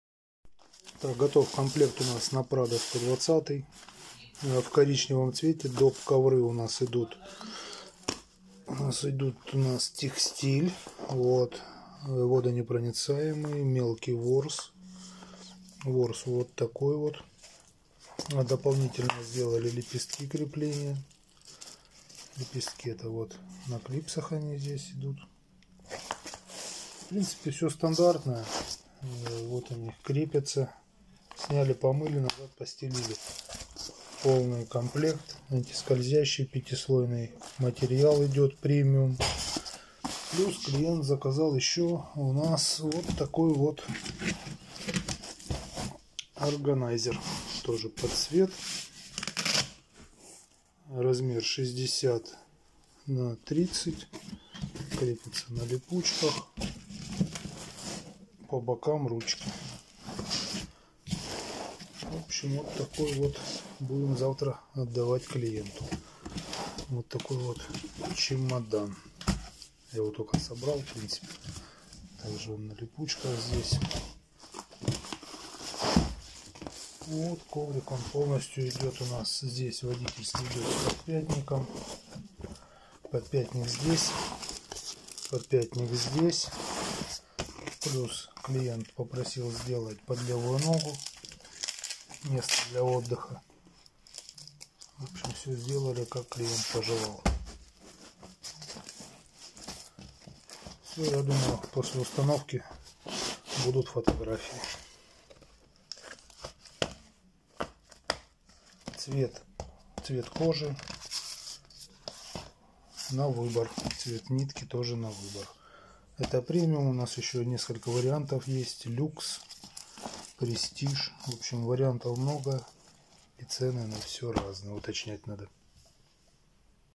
3д коврики, экокожа + доп ковры пвх мини, на Prado 120.
📌 Пошив 3d ковров в салон на TOYOTA LAND CRUISER PRADO 120.
ДОП КОВРИКИ ПВХ МИНИ
ПРИ ЗАКАЗЕ ЦВЕТ МАТЕРИАЛА И СТРОЧКИ НА ВЫБОР.